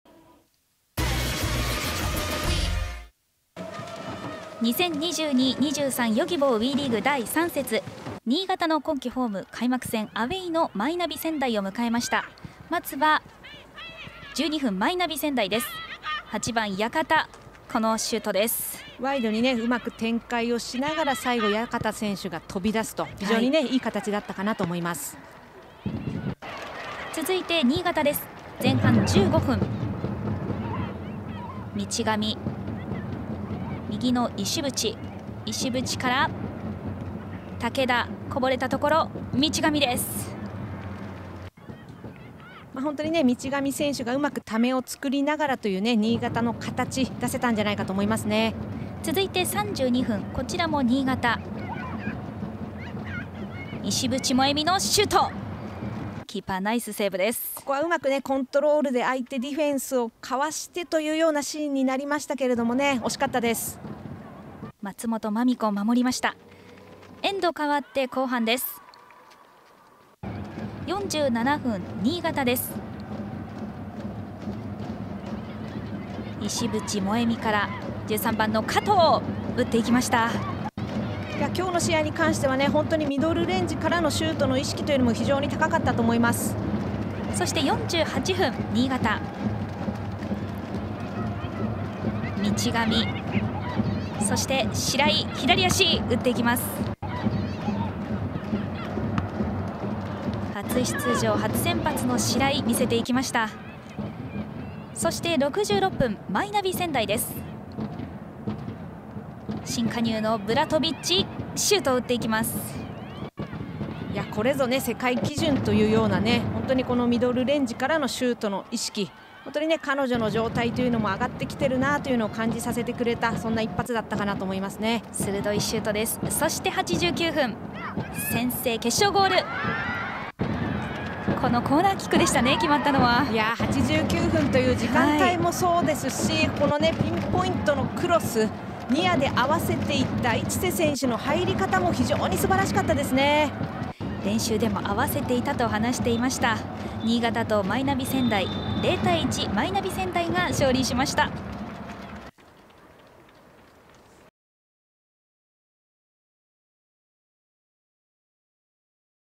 【公式】ハイライト：アルビレックス新潟レディース vs マイナビ仙台レディース【Yogibo WEリーグ 第3節 2022.11.5】
【Yogibo WEリーグ 第3節 ハイライト動画】

0:00 0-0 試合開始
2:05 0-1 後半44分 市瀬菜々(マイ仙台)

2022年11月5日（土）14時キックオフ
会場：デンカビッグスワンスタジアム

「DAZN」で全試合生中継！